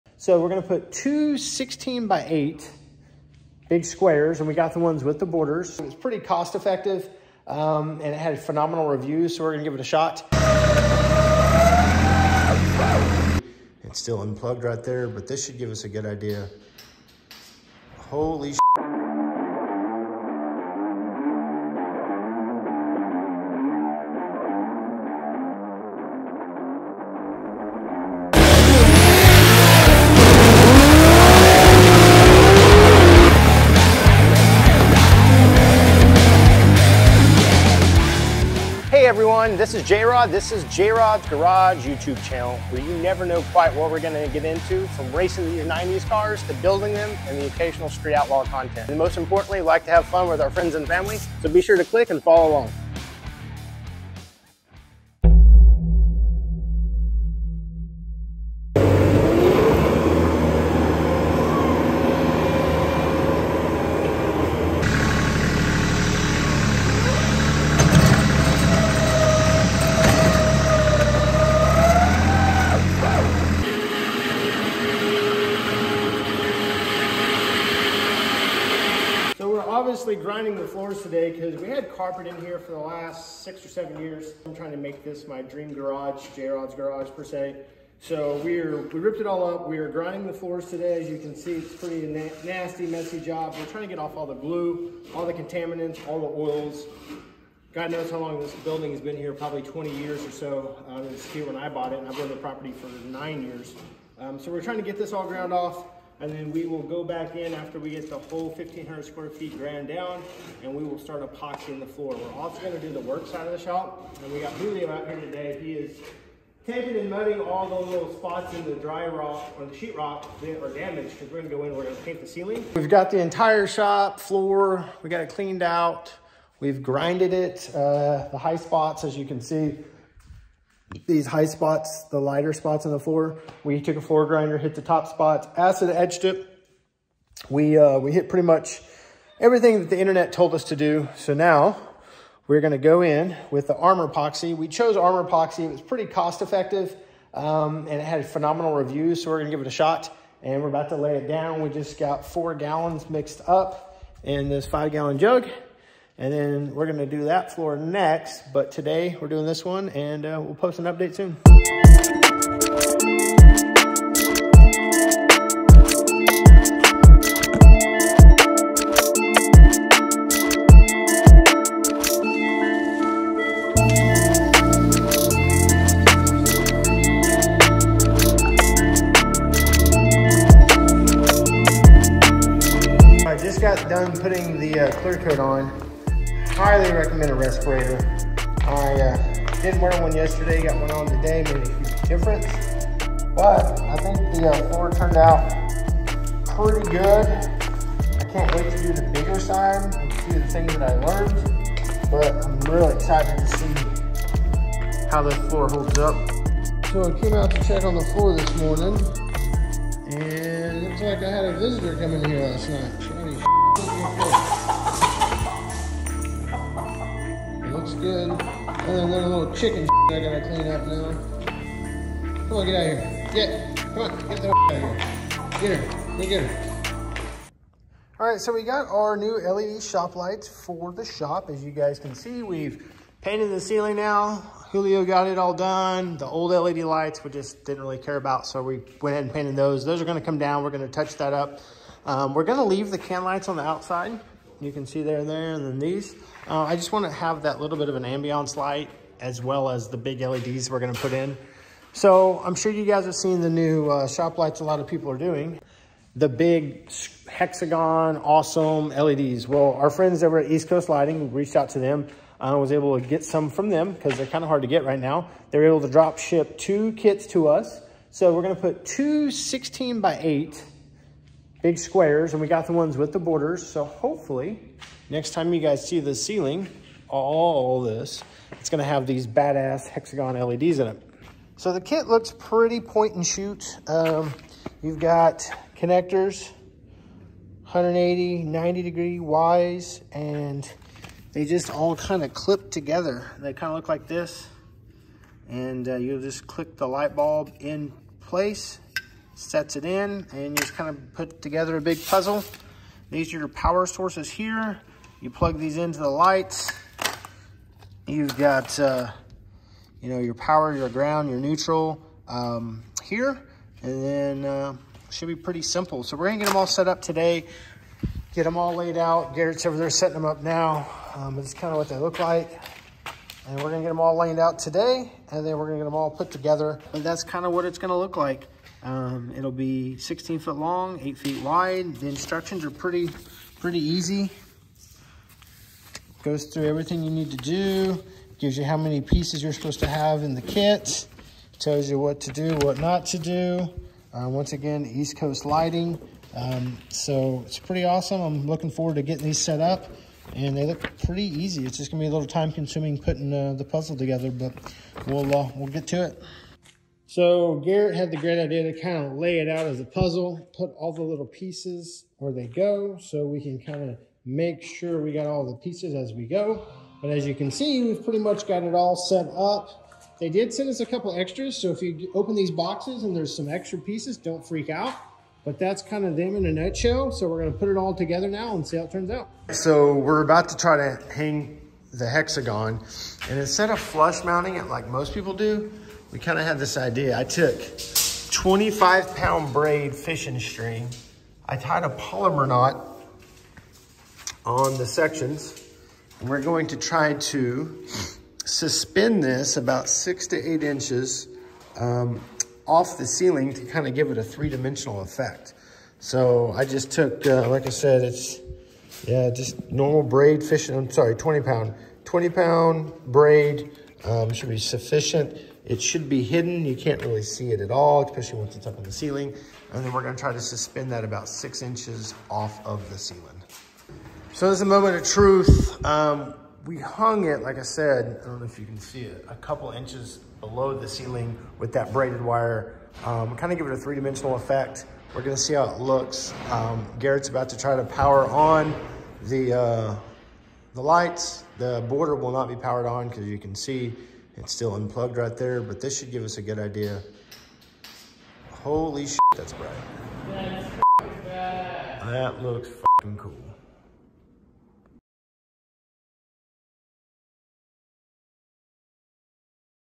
Building My DREAM GARAGE! Complete Makeover! Part 1
Today we start building my dream garage! This is part one of my garage transformation. We ripped out the old carpet, sanded and ground the floors, applied new floor coating as well as electrical work, new doors and lights! There's still quite a lot left to complete but it's already transforming into exactly what I imagined! Installing epoxy floors from Armorpoxy was a GAME CHANGER!